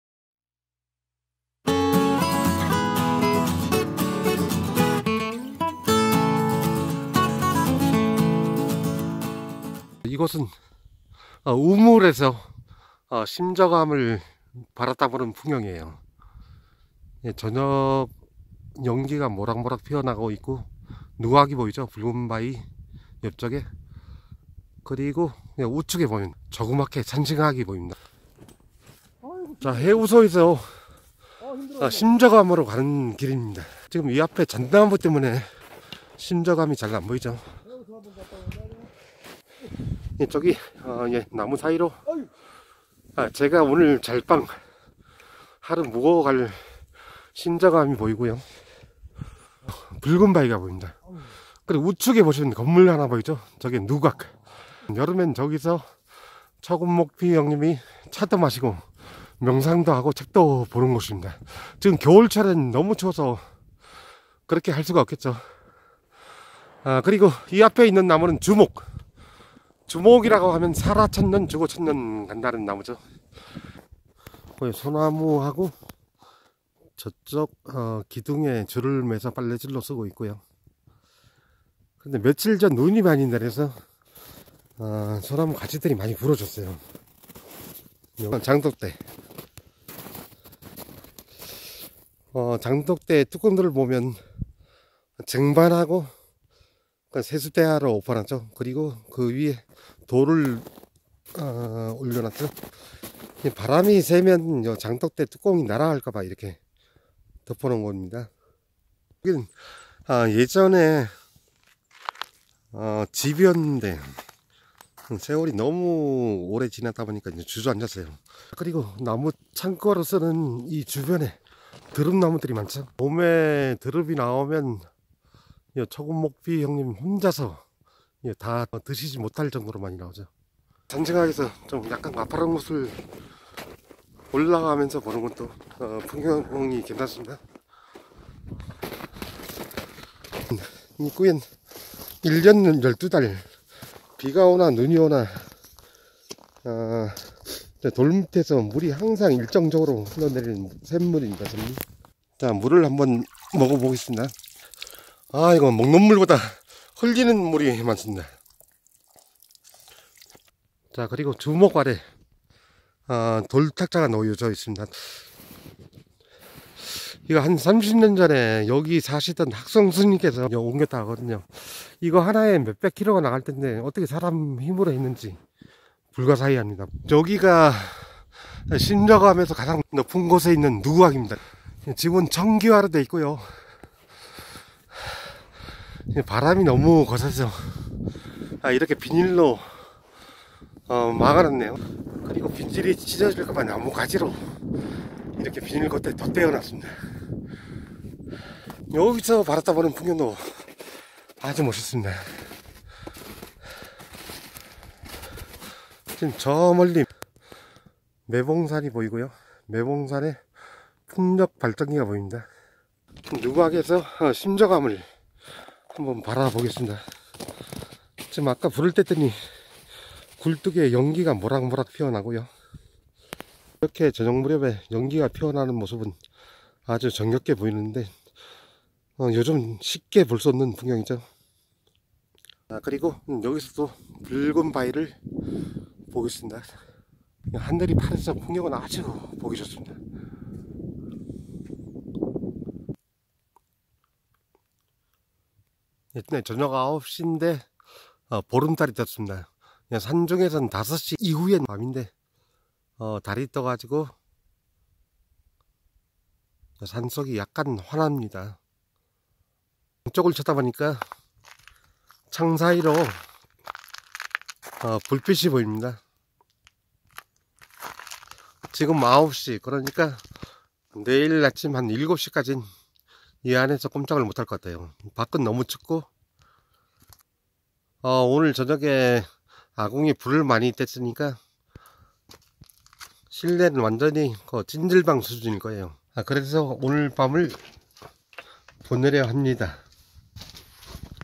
최고의 오지 심적암에 도착하다. 자연인을 찾아서 2편.
우리나라 최고의 오지 심적암에 도착하여 주변을 살펴 본 후  보름달을 보고 잠을 자기 위해 방안으로 들어 갑니다.
약초꾼 이형설 (010~7123~2294번)